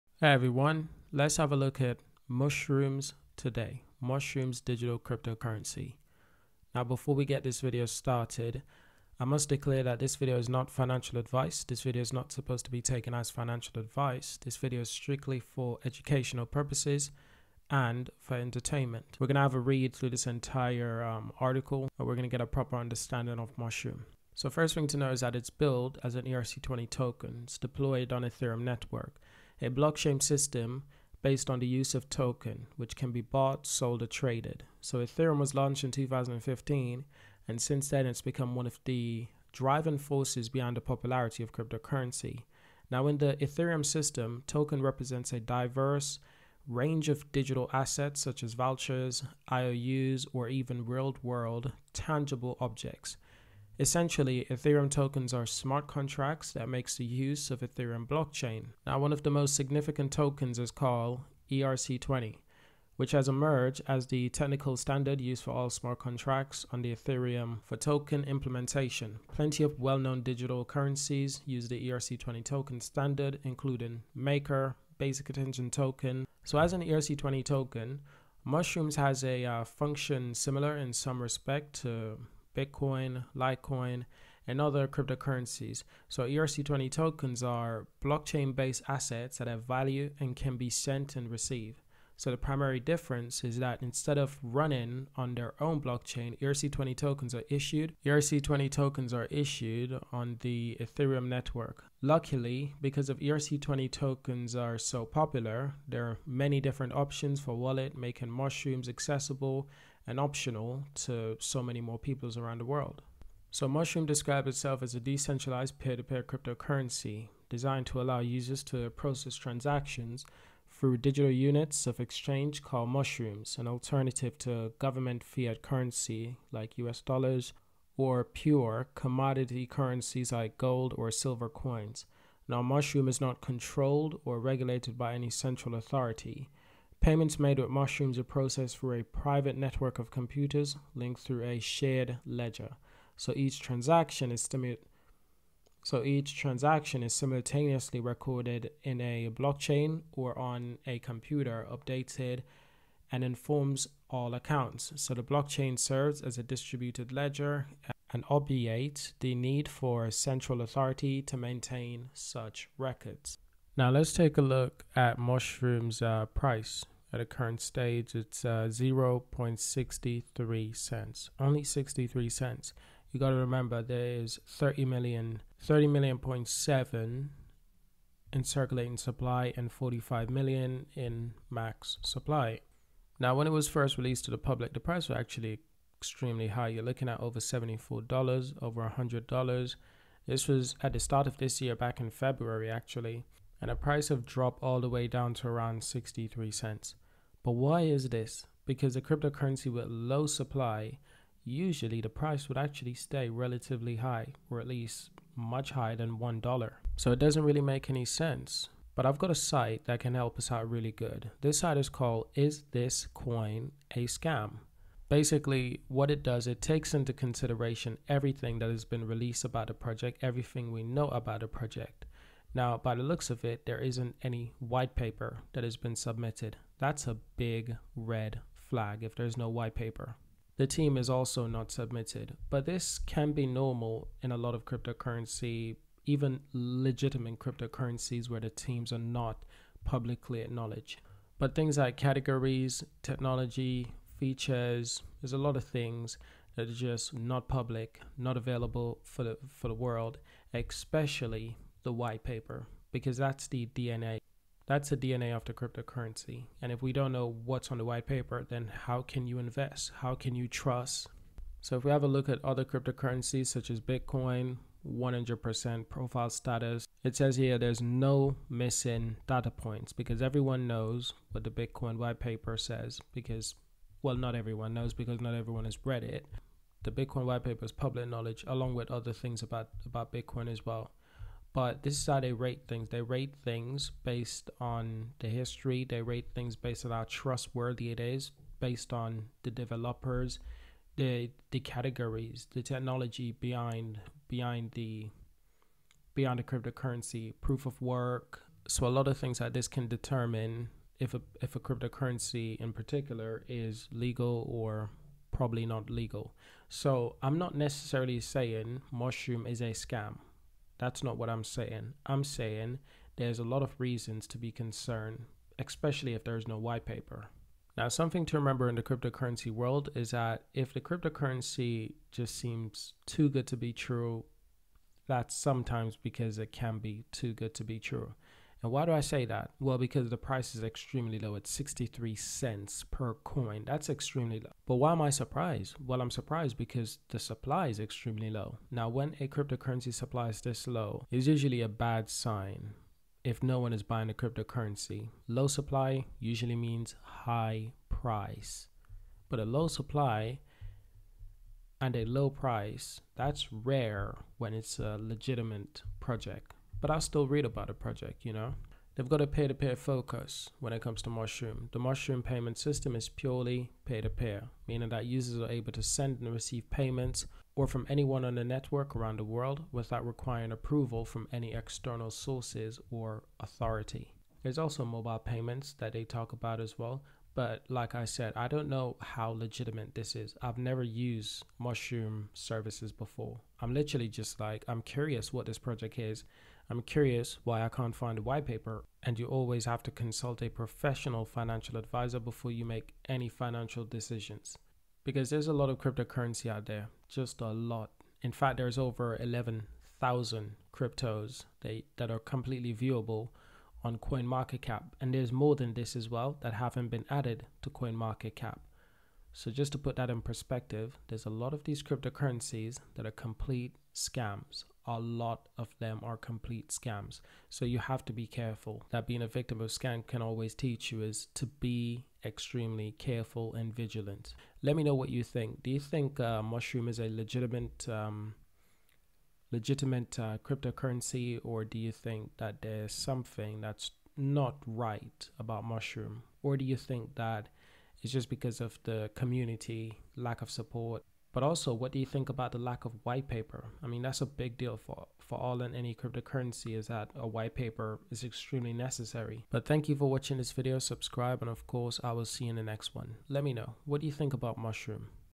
What is Mushroom? Why is The Price So Low! Is Mushroom A Scam or 100X Crypto?
Article 1 -

All information is meant for public educational puposes.